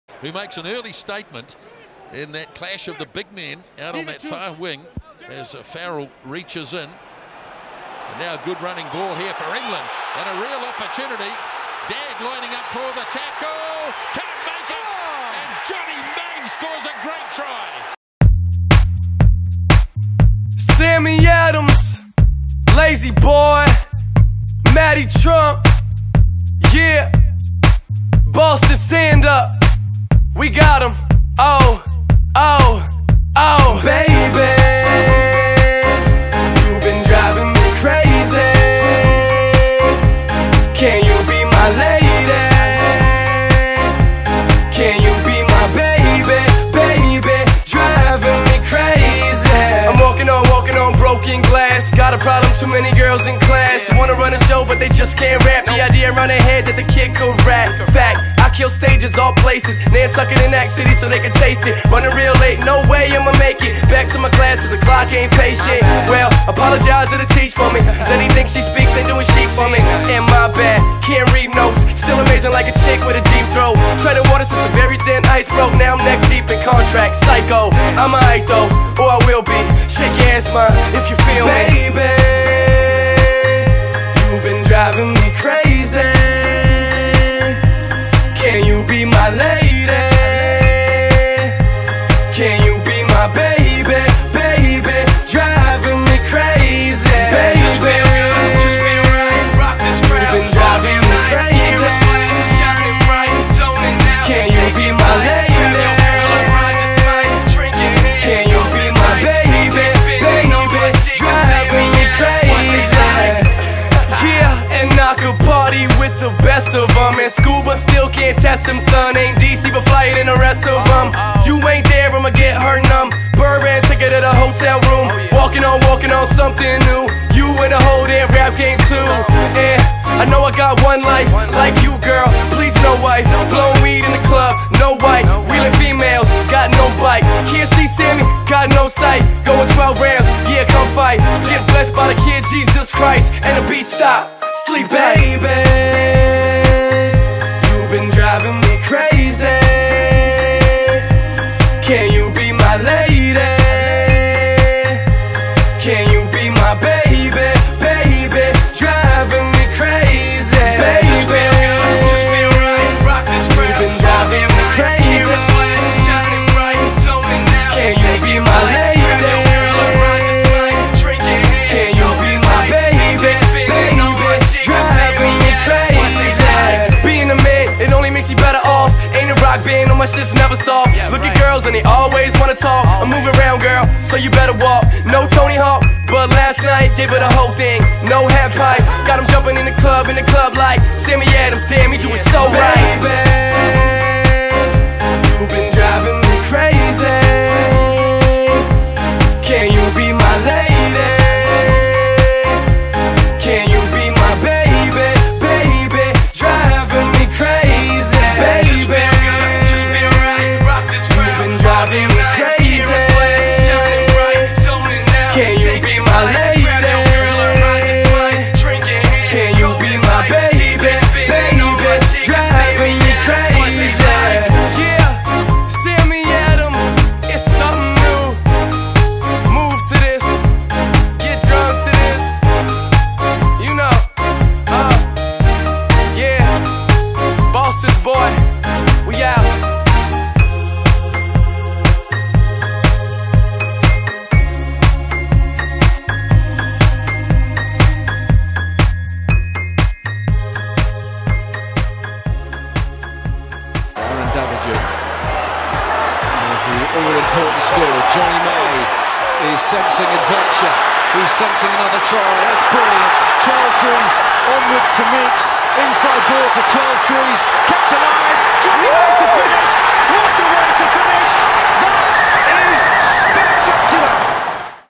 Jonny May Tribute - Blink and You'll Miss Him
Jonathan James May (born 1 April 1990) is a professional rugby union player for Leicester Tigers in Premiership Rugby and for the England national side; between 2009–17 he played 121 games for Gloucester. He plays as a winger and is known for his electrifying pace and finishing skills.